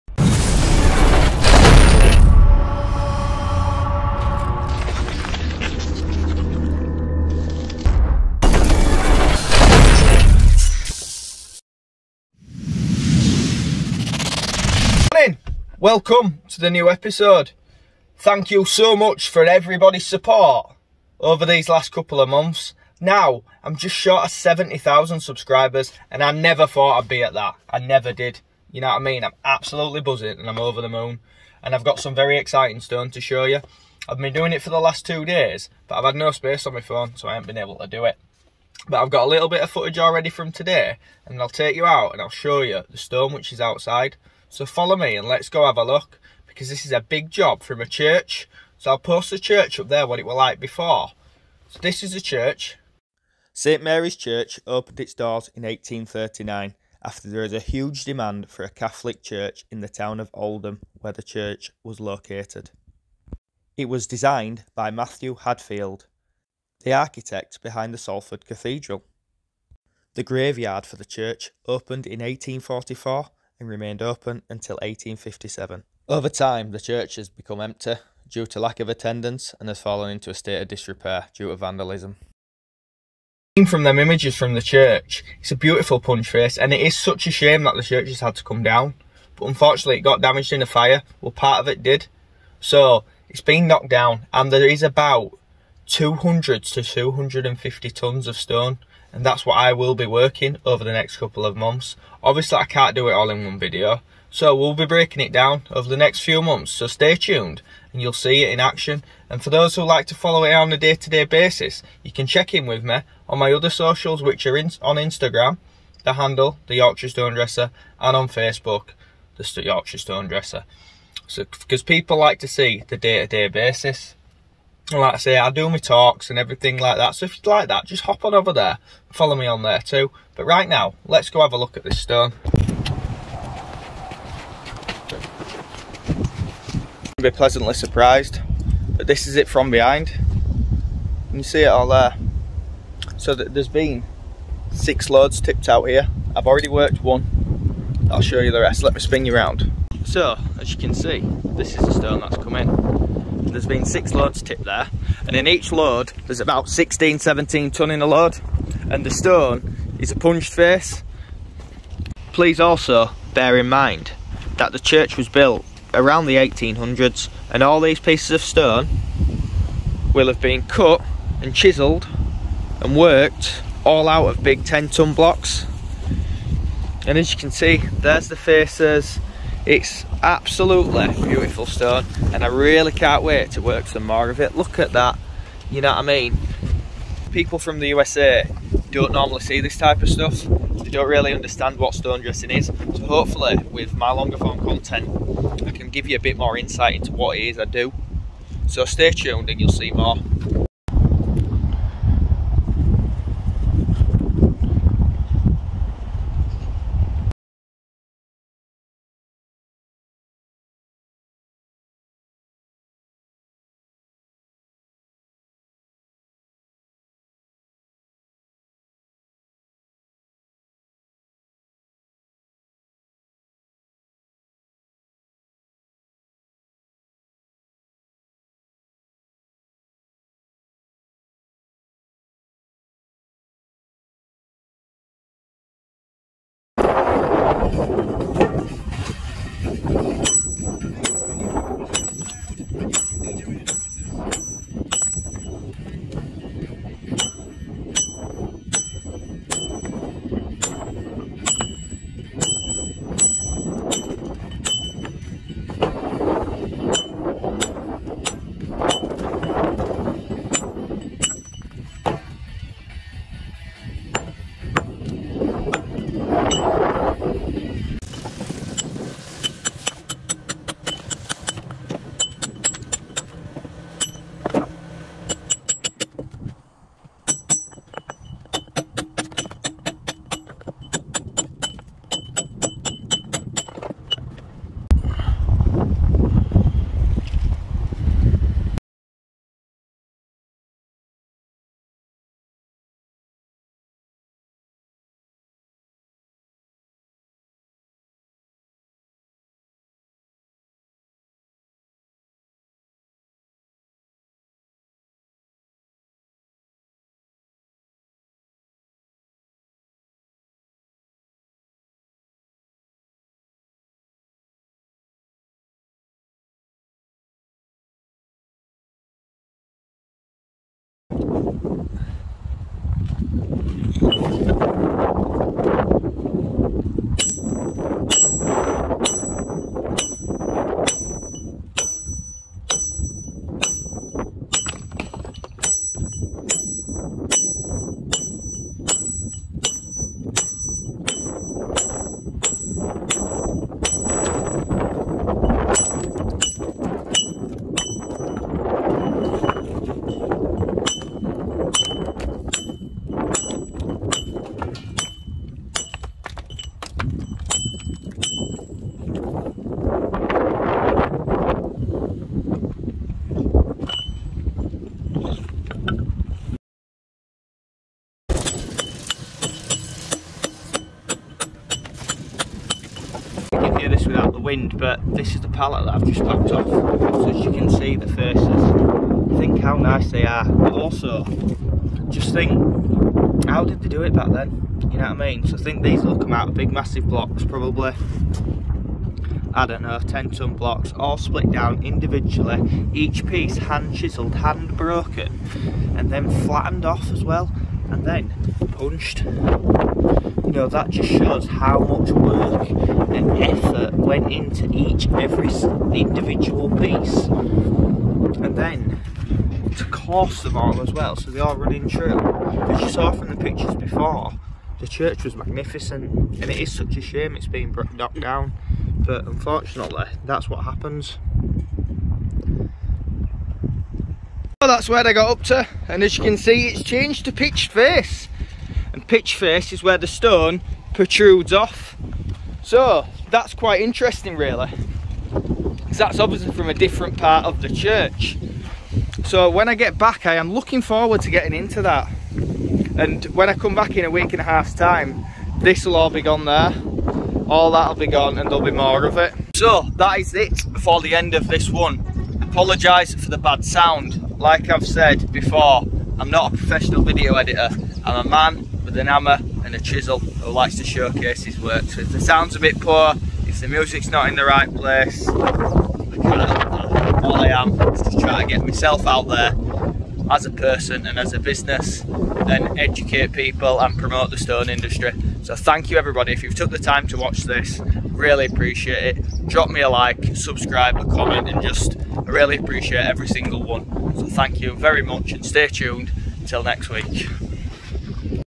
Church Project - The beginning
In this episode you will see me beginning the start of a big project for a regular client.

St Mary's Church in Oldham was opened in 1839 in response to demand for a Catholic church in the Oldham area and was designed by Matthew Hadfield, the architect behind Salford Cathedral.

It has suffered from high-level water ingress and dry and wet rot to the structural timbers

Over the coming months you will see me working the stone.

you can follow my progress on my other socials